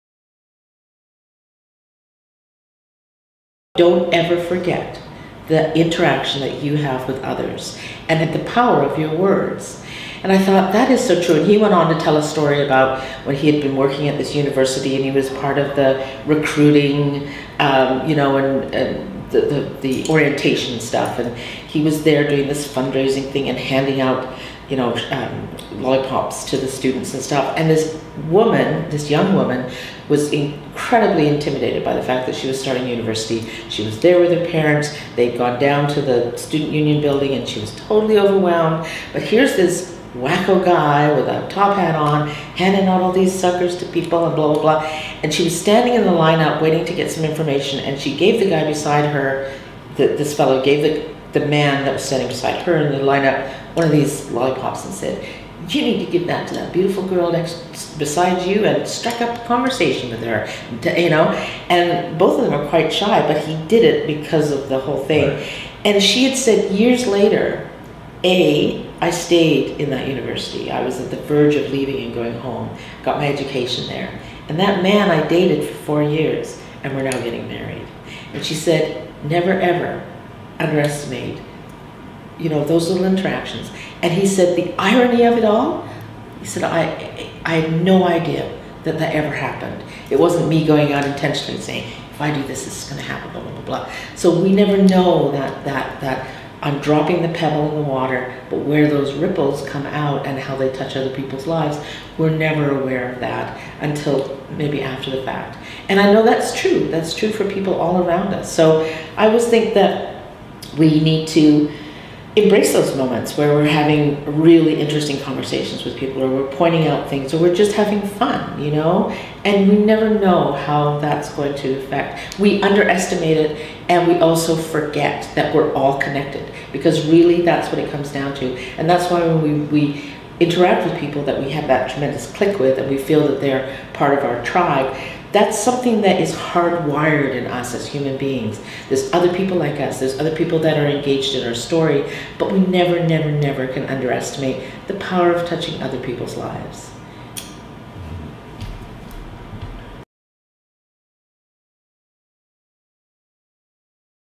Power of little interactions
Tracy Yerrell  -- inspired by a TED talk, shares the importance of the little interactions in our days